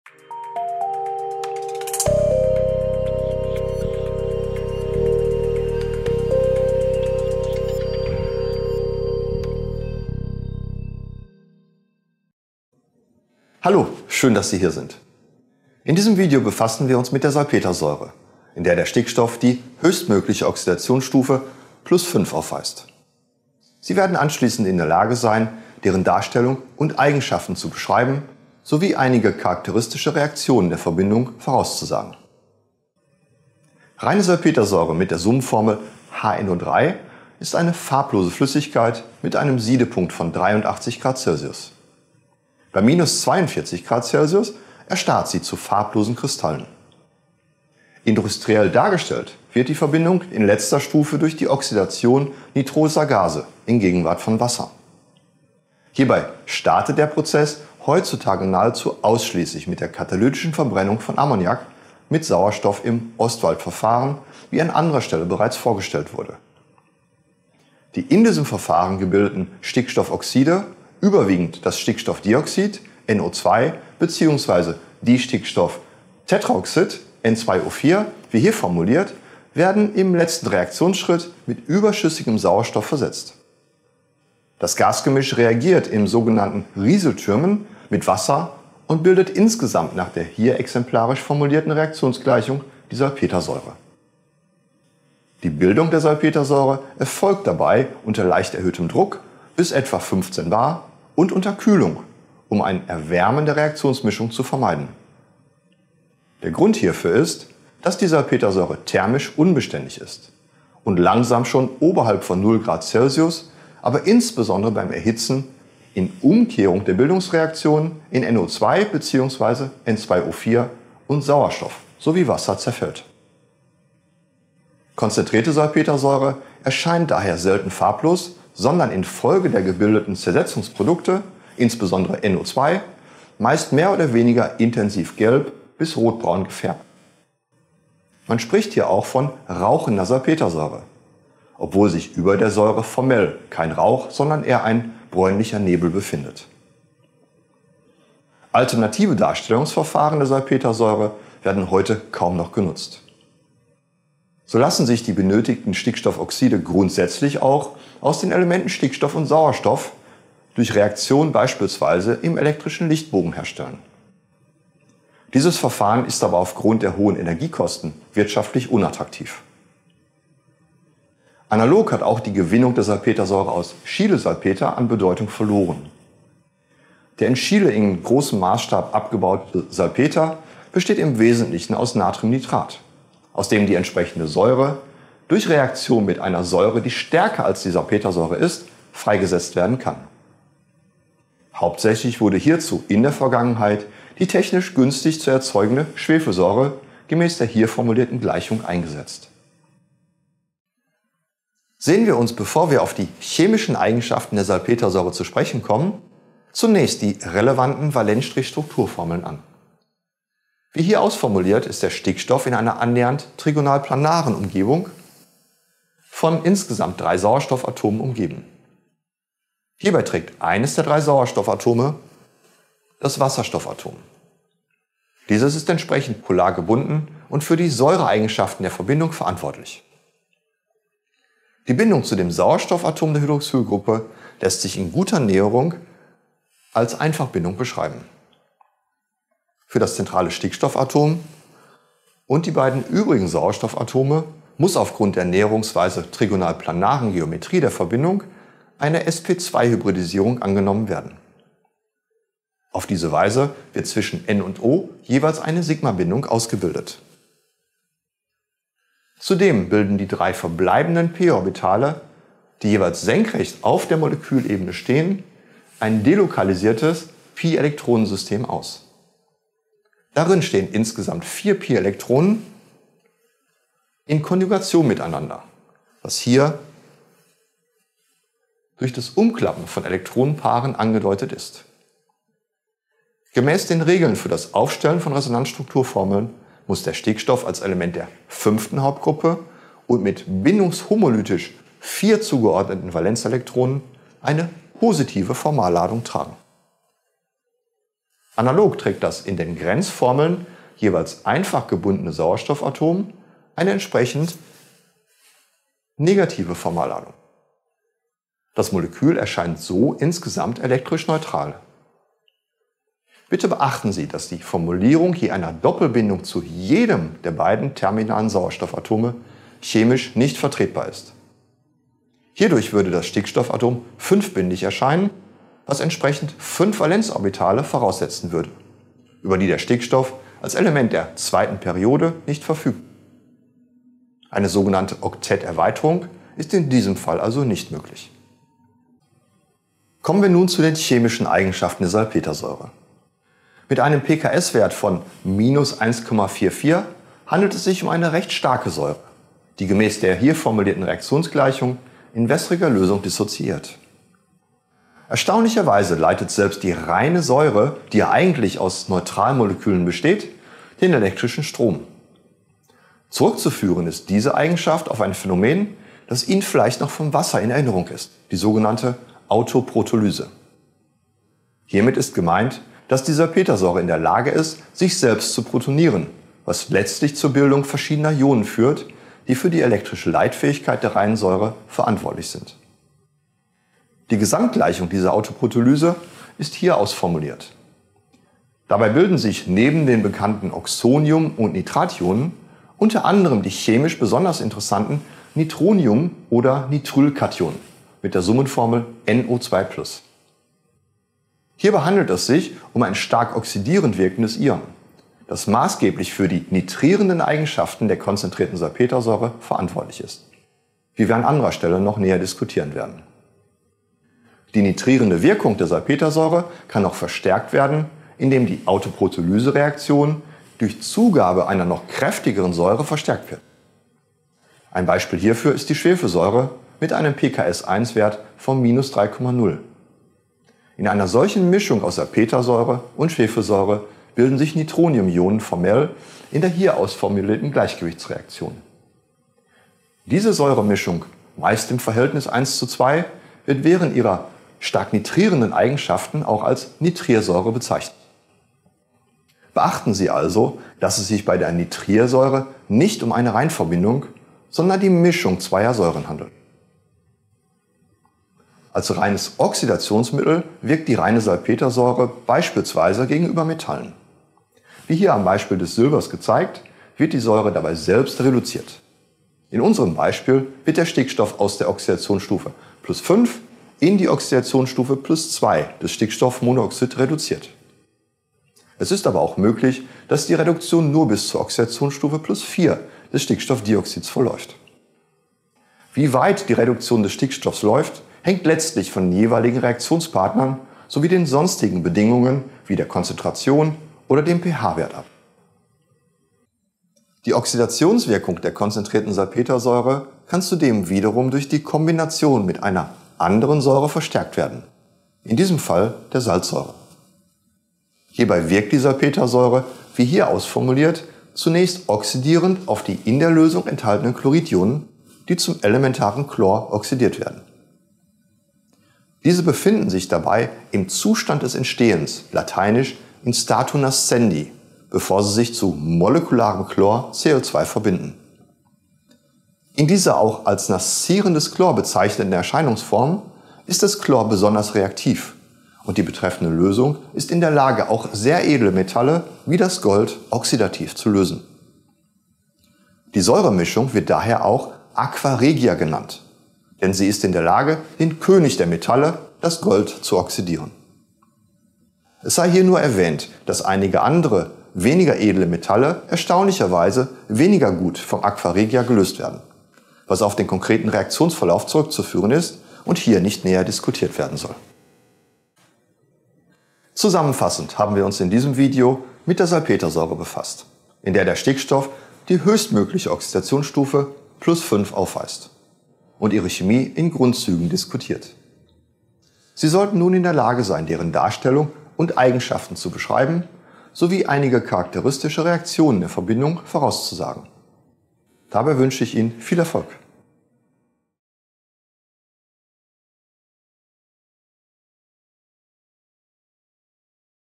Salpetersäure - Anorganische Chemie
Die physikalischen und chemischen Eigenschaften der Salpetersäure werden diskutiert. Hiervon werden einige technische Anwendungen sowie Konsequenzen für das Grillen von Fleisch abgeleitet.